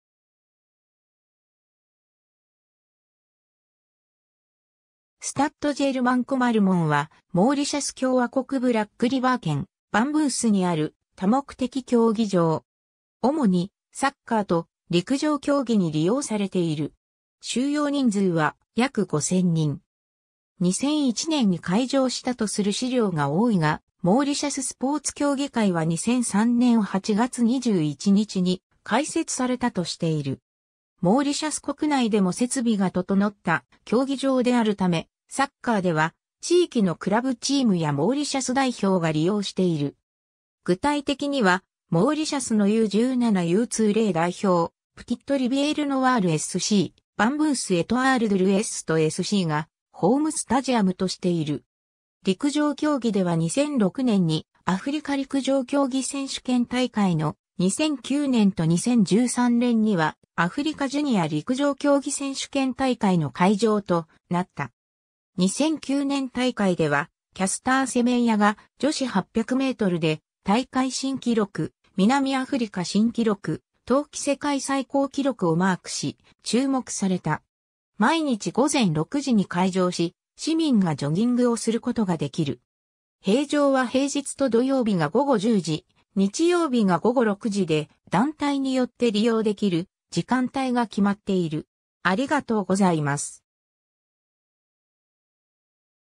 スタッド・ジェルマン・コマルモン
スタッド・ジェルマン・コマルモン（フランス語: Stade Germain Comarmond）は、モーリシャス共和国ブラックリバー県バンブースにある多目的競技場。
主にサッカーと陸上競技に利用されている。
収容人数は約5,000人。
2001年に開場したとする資料が多いが、モーリシャススポーツ協議会は2003年8月21日に開設されたとしている。
 モーリシャス国内でも設備が整った競技場であるため、サッカーでは地域のクラブチームやモーリシャス代表が利用している。
具体的には、モーリシャスのU17・U20代表、プティット・リヴィエール・ノワールSC、バンブース・エトアール・ドゥ・ルエストSCがホームスタジアムとしている。
陸上競技では2006年にアフリカ陸上競技選手権大会の、2009年と2013年にはアフリカジュニア陸上競技選手権大会の会場となった。
2009年大会ではキャスター・セメンヤが女子800mで大会新記録、南アフリカ新記録、当季世界最高記録をマークし、注目された。
 毎日午前6時に開場し、市民がジョギングをすることができる。
閉場は平日と土曜日が午後10時、日曜日が午後6時で、団体によって利用できる時間帯が決まっている。